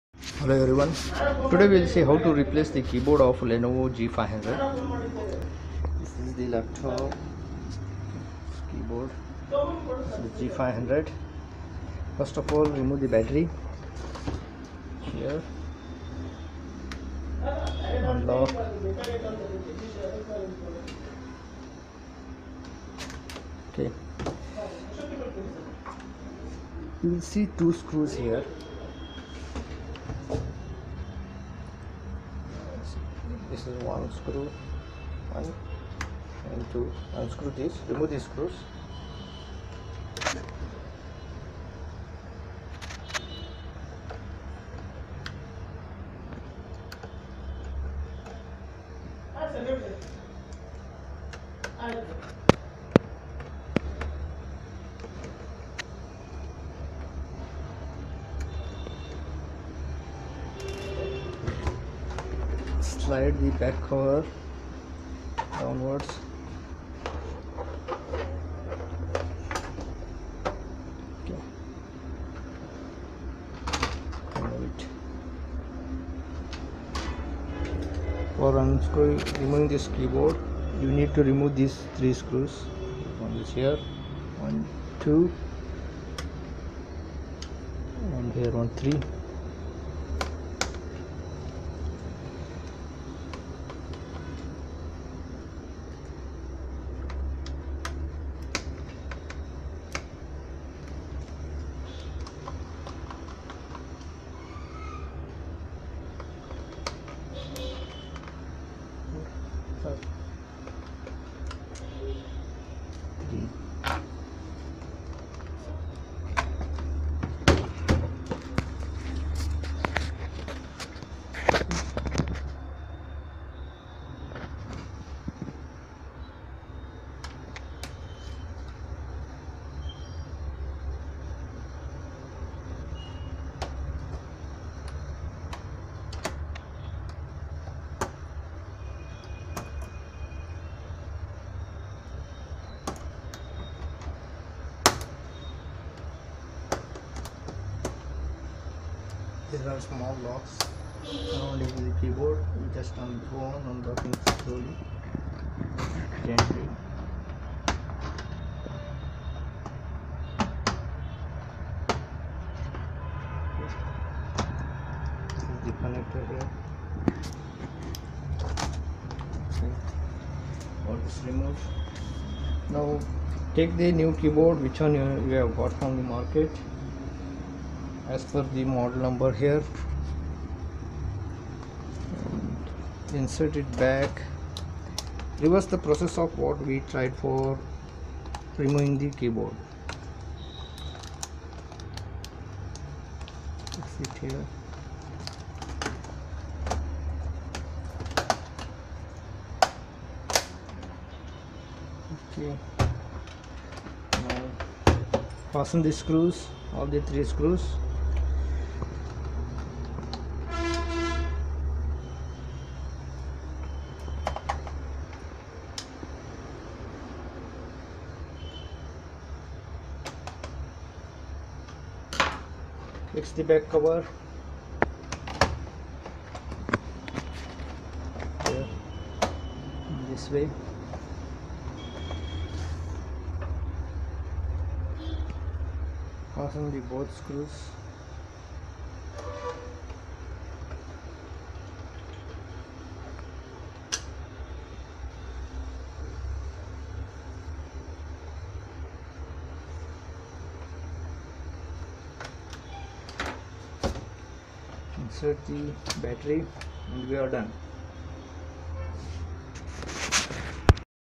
Lenovo G500 Keyboard replacement || changing.
This video shows simple way of changing lenovo g500 keyboard.